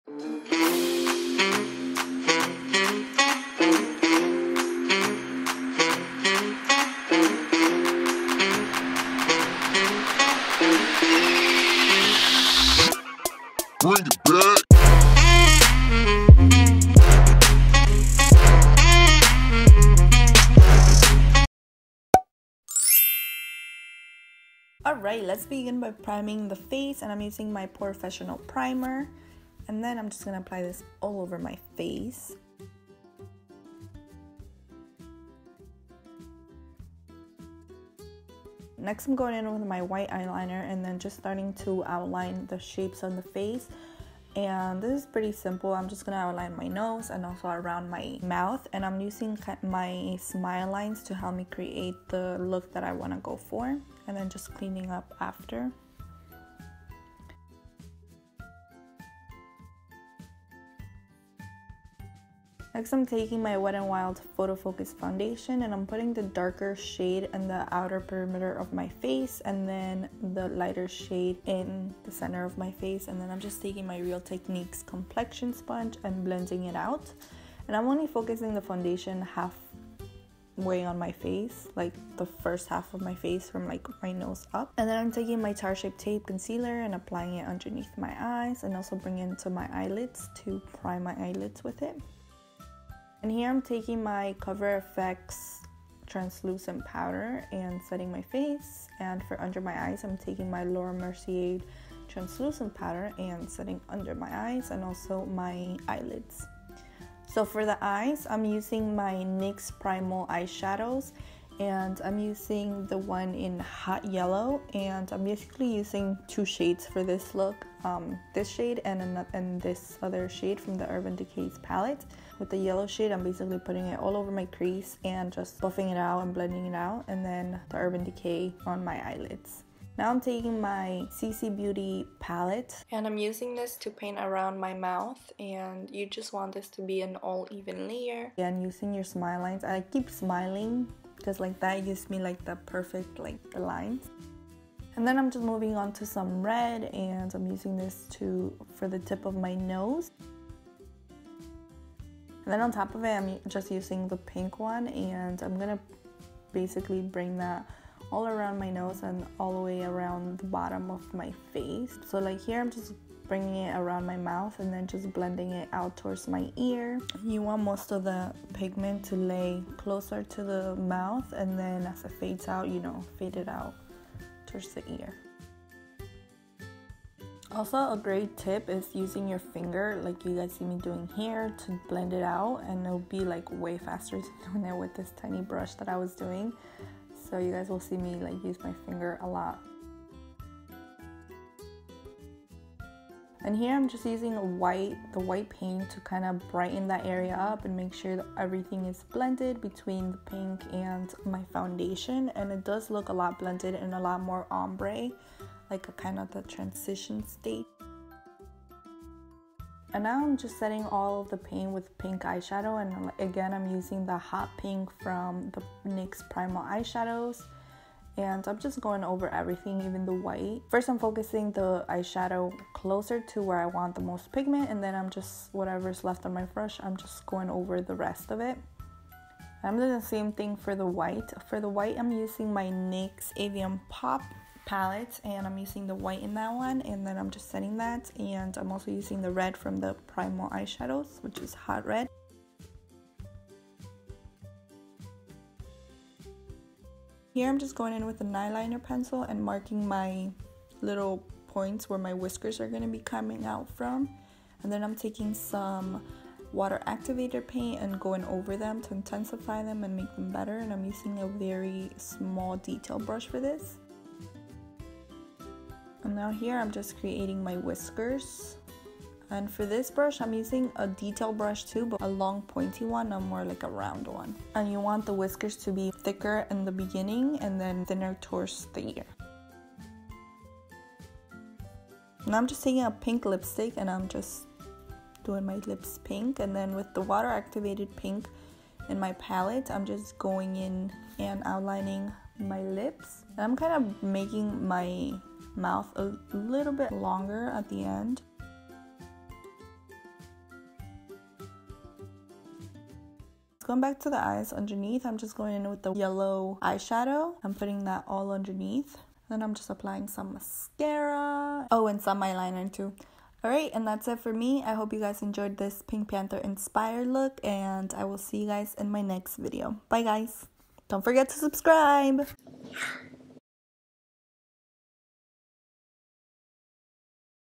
Easy Pink Panther Look | Halloween 2017 | Virginiaaaxo
( WATCH IN HD) Hey Guys, here is a very easy halloween makeup look. It's Pink Panther inspired and I hope you guys love it.

PRODUCTS USED:

Wet n Wild Photo Focus Foundation | Golden Beige | Desert Beige
Tate Shape Tape Concealer | Medium
CC Beauty Face Paint
NYX Primal Eyeshadow | Hot Yellow | Hot Red | Hot Pink
NYX Avant Pop Palette

Until Next Time Dollies
XOXO 💕